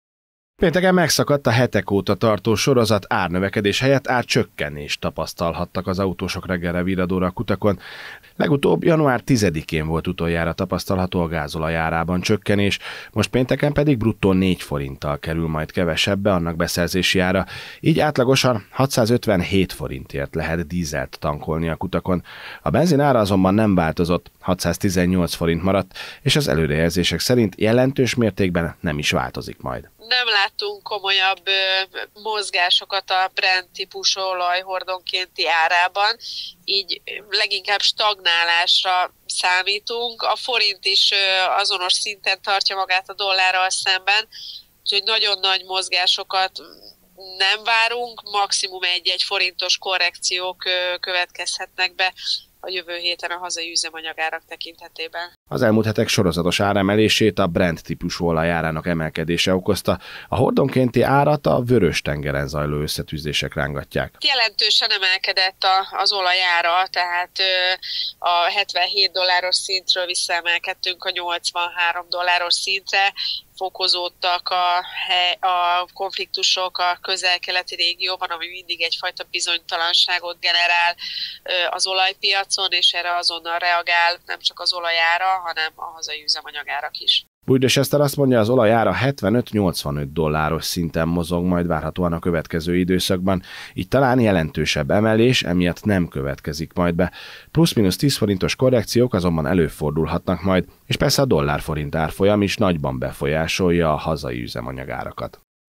EMELKEDÉS UTÁN NÉMI CSÖKKENÉS
Hosszú idő óta újra csökkent péntektől a gázolaj nagykereskedelmi ára. Legutóbb január 10-én tapasztaltunk a gázolaj árában csökkenést, most péntektől pedig bruttó 4 forinttal kerül kevesebbe annak beszerzési ára. A benzin ára azonban a héten már nem változik.